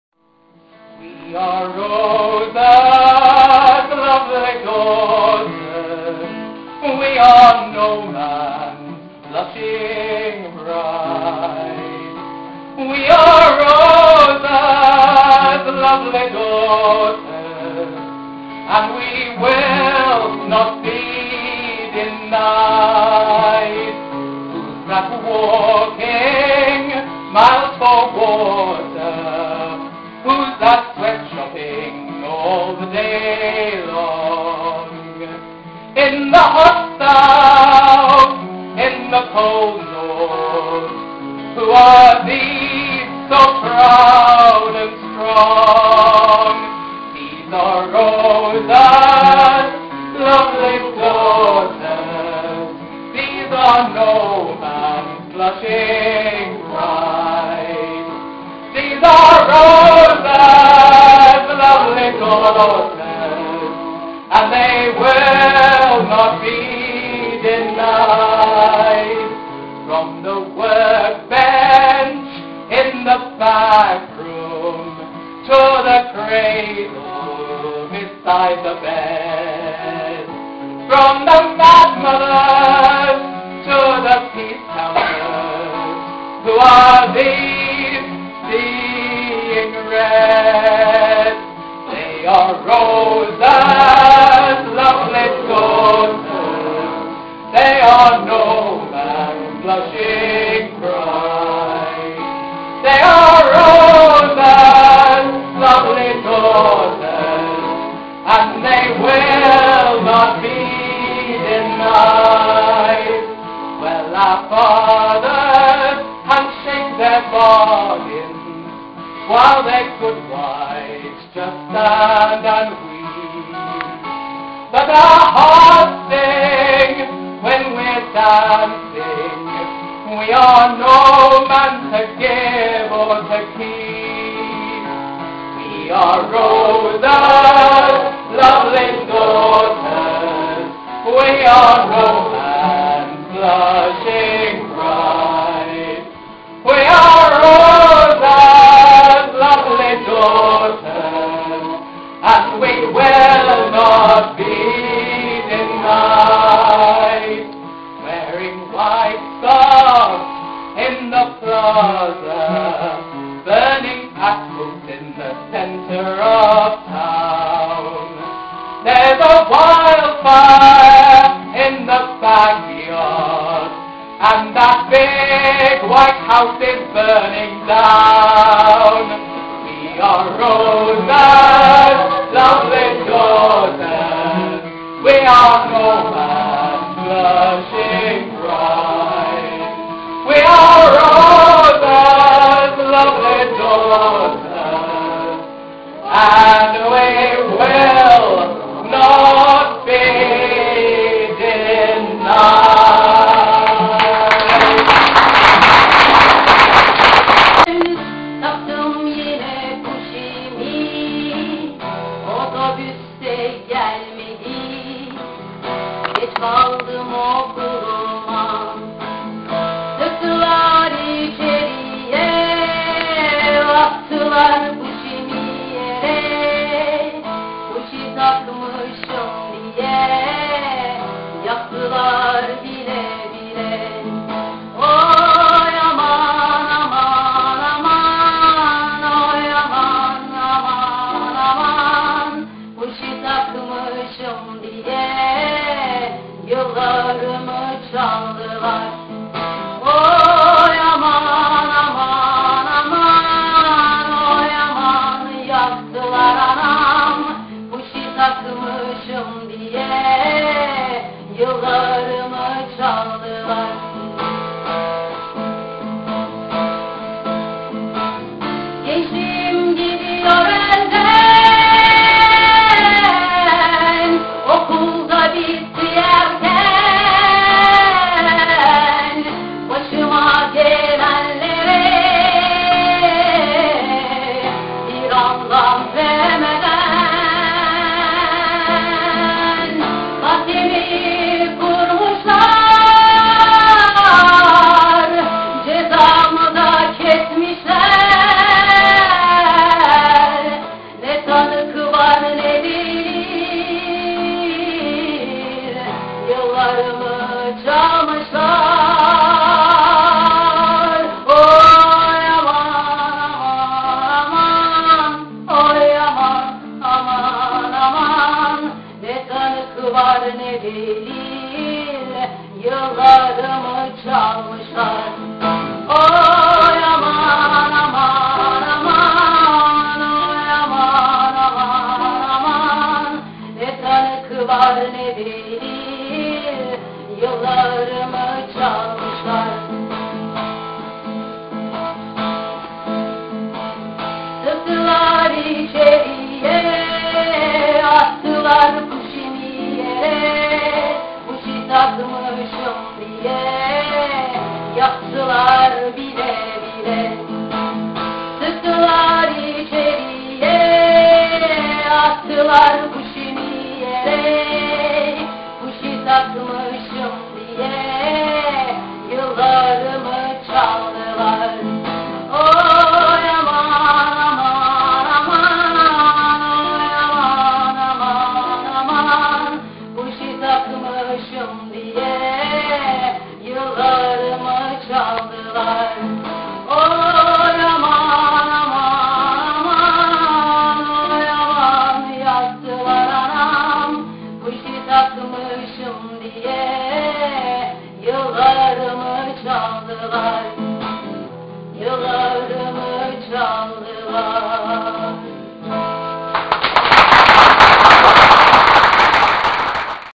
Międzynarodowy Dzień Kobiet - 2012 r. (Londyn)
Fragment występów z okazji Międzynarodowego Dnia Kobiet 8 marca. Nagranie wykonane w Marx Memorial Library w Londynie w 2012 roku.